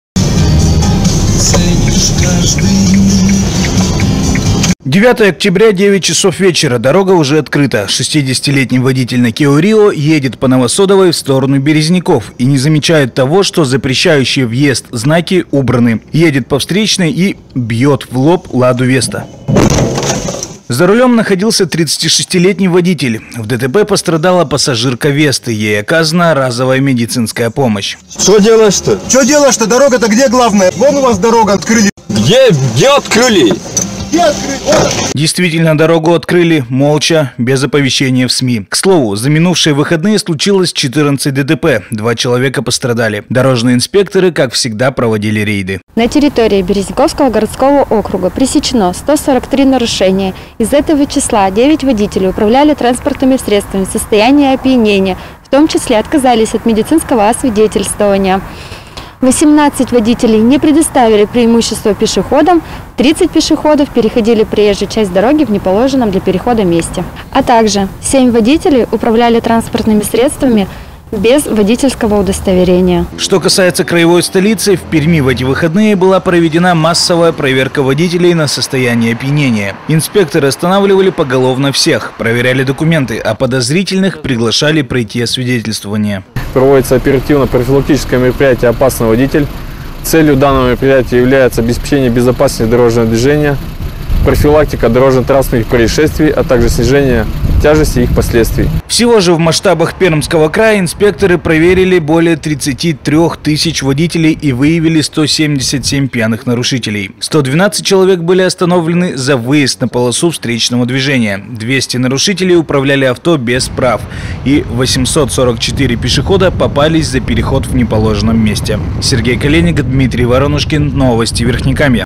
ДТП
Открыли без предупреждения. После длительного ремонта дорогу по улице Новосодовая открыли в оба направления. Но не все водители были рады этому событию. Мужчина на автомобиле корейской фирмы по привычке поехал по встречной и стал виновником лобового ДТП. Сергей Коленик продолжит.